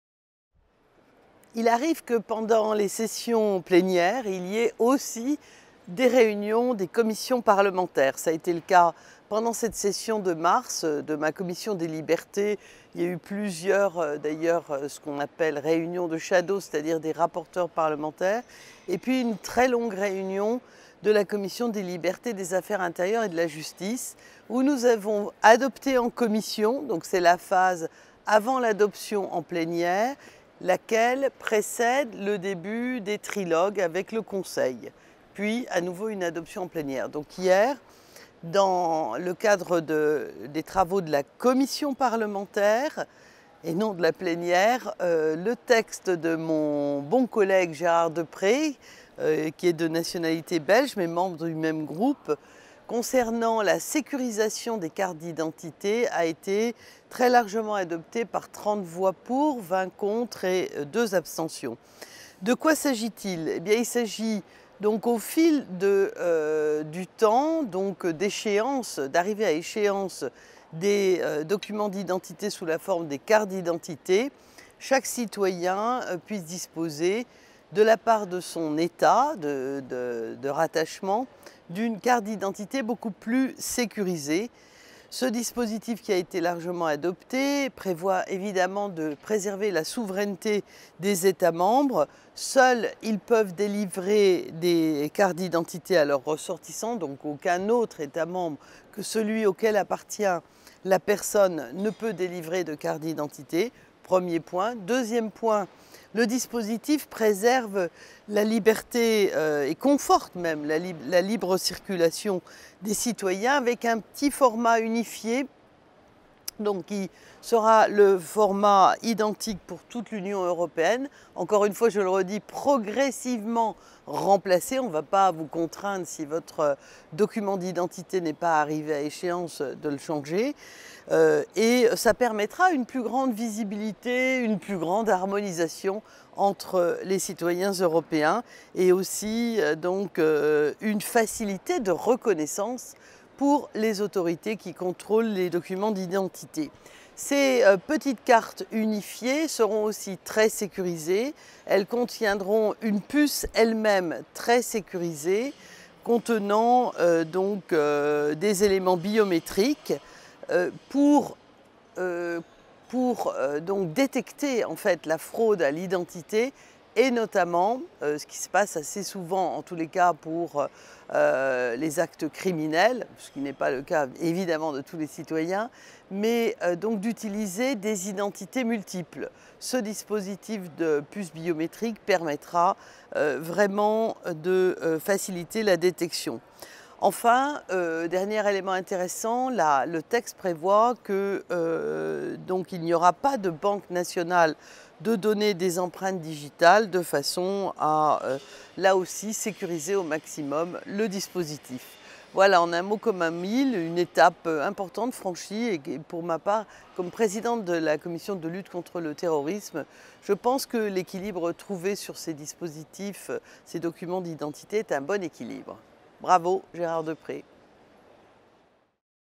Une carte d’identité plus sûre en Europe
En marge de la session plénière, Nathalie Griesbeck a participé à une réunion de la commission des Liberté civiles (LIBE) qui s’est déroulée le lundi 11 mars. Dans le cadre de cette réunion, les députés membres de la commission ont adopté à une forte majorité le Règlement sur la sécurisation des cartes d'identité - un dossier sur lequel le Parlement est représenté par Gérard Deprez, député européen belge, membre du même groupe parlementaire (ADLE) et du même Parti politique européen (PDE) que Nathalie Griesbeck !